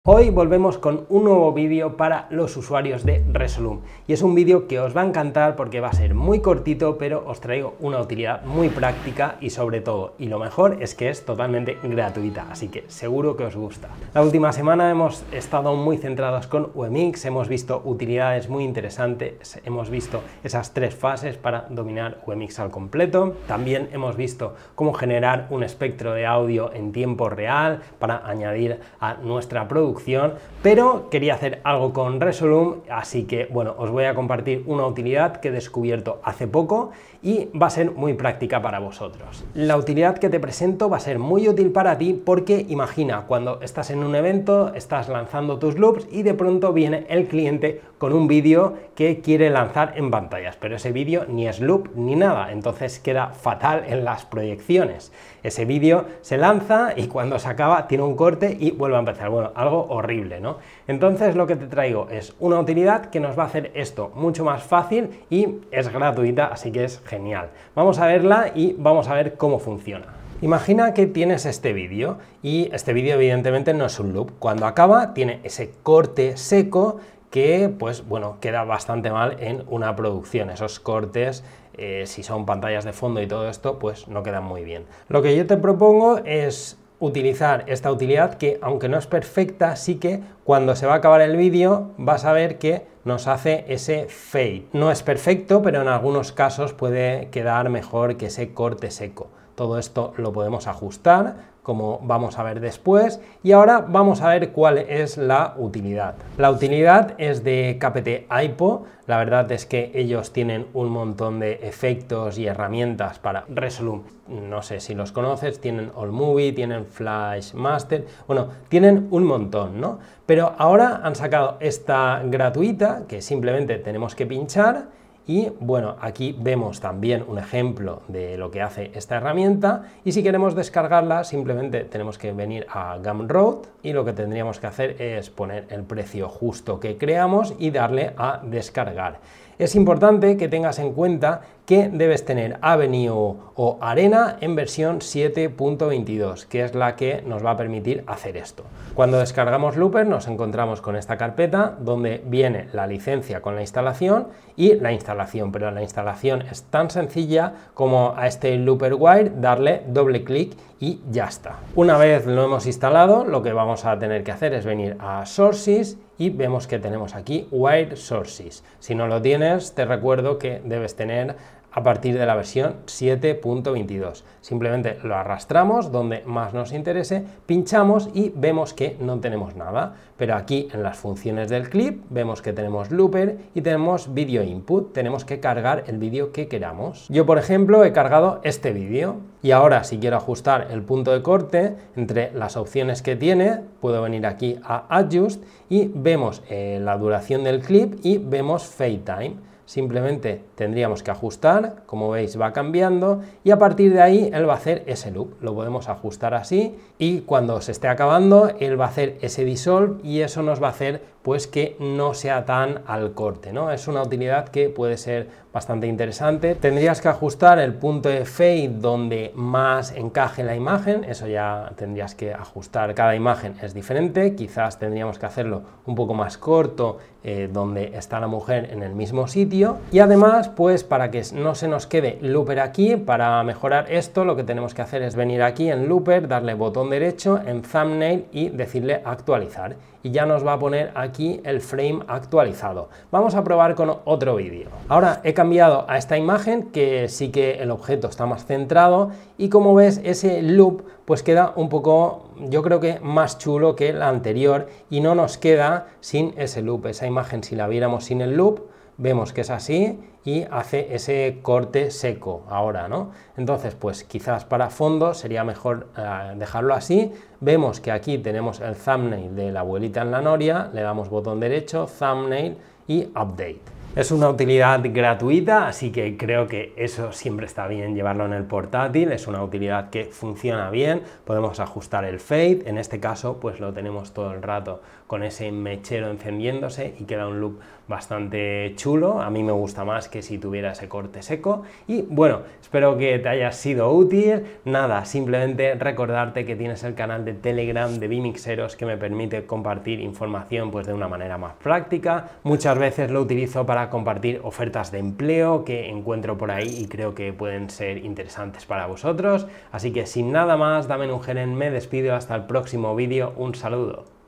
FX para RESOLUME ▶ Looper VJ Utilidad GRATIS para video jockey
FX LOOPER para Resolume 🧡 Utilidad Gratuita muy interesante para Resolume

✅𝗠𝗶𝘀 𝗘𝗤𝗨𝗜𝗣𝗢𝗦:
👉 Monitor HDMI-SDI
👉 Tienda Amazon

⚡️¿𝗛𝗮𝗯𝗹𝗮𝗺𝗼𝘀?

▼▼ÍNDICE▼▼
00:00 Intro
00:14 Semana vMix
00:39 Utilidad Gratis para Resolume